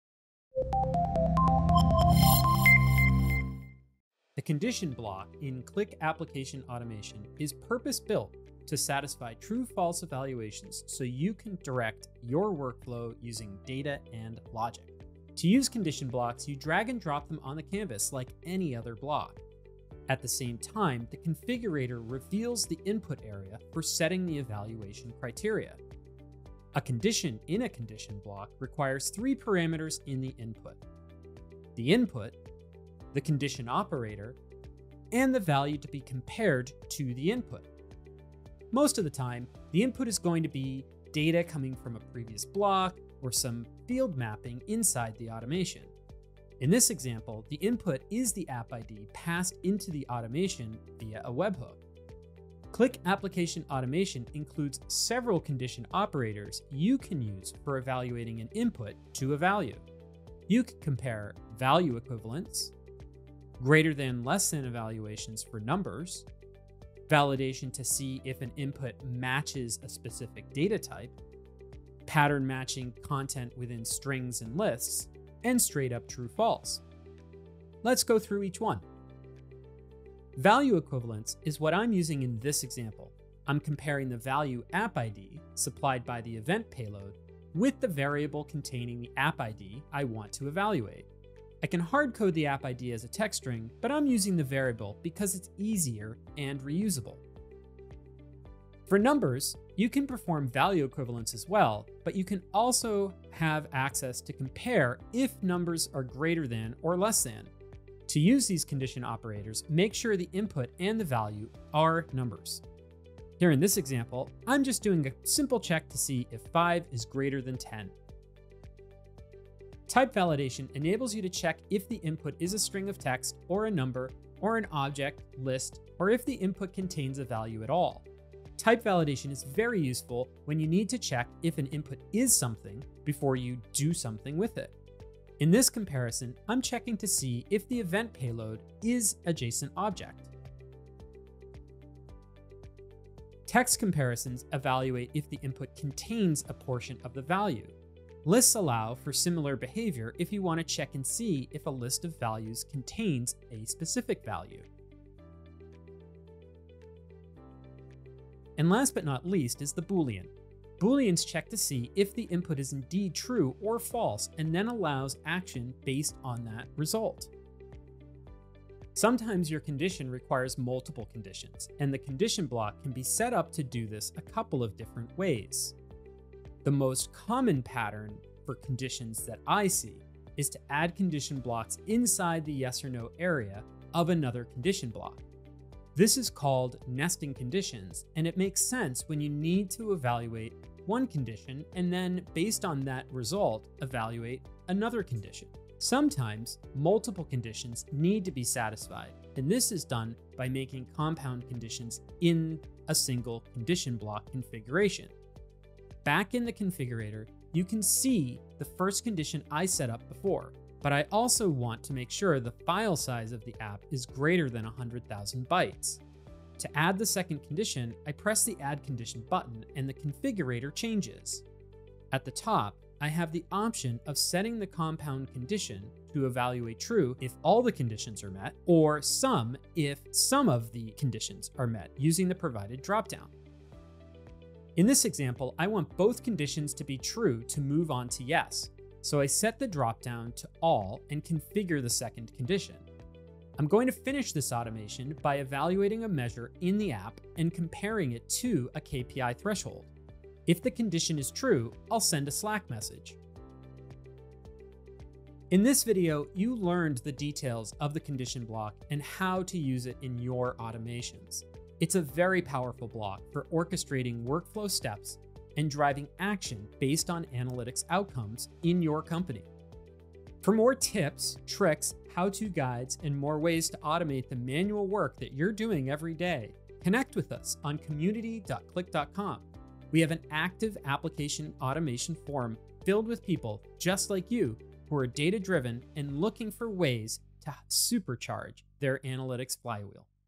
Get to know Condition Block - Qlik Automate
Learn how to use the Condition block in Qlik Automate to create intelligent, data-driven workflows. This tutorial walks through how to configure evaluations using a variety of operators like equals, greater than, contains, type checks, and booleans. You’ll also explore how to build nested and compound conditions, and apply logic to real-world automations like KPI evaluation or webhook-driven workflows. Ideal for automation creators looking to build dynamic, rule-based decision trees.

🔍 00:00 — What Is the Condition Block?
📦 00:15 — How to Add and Configure a Condition Block
⚙️ 00:26 — Three Inputs: Field, Operator, Value
🔗 00:38 — Using Field Mapping as Input
🧮 00:52 — Common Operators and Their Use Cases
📊 01:00 — Example: Value Equivalence with App ID
🔢 01:38 — Number Comparison: Greater Than / Less Than
📐 02:03 — Type Validation: Is It a String, Object, or List?
📝 02:27 — String and List Matching
✅ 02:40 — Boolean Logic: True/False Conditions
🔄 02:48 — Nesting Conditions (Yes/No Branches)
🧩 03:05 — Compound Conditions in One Block
📚 03:25 — Setting 'All' vs 'Some' in Compound Logic
📏 03:58 — Example: File Size and ID Match Together
📈 04:07 — KPI Evaluation with Threshold Check
📣 04:13 — Triggering Slack Notifications on True Conditions
🎓 04:18 — Summary: What You Learned
🚀 04:21 — Use Cases for Conditions in Workflow Automation
🌐 04:30 — Join the Qlik Community for More Tips & Guides